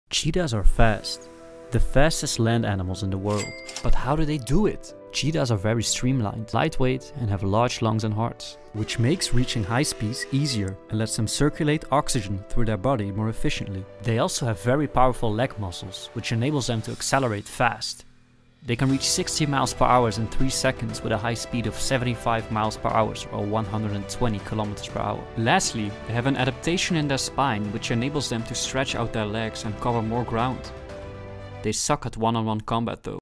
The speed of cheetah's explained!
In this short the speed of cheetah's gets explained in 60 seconds or less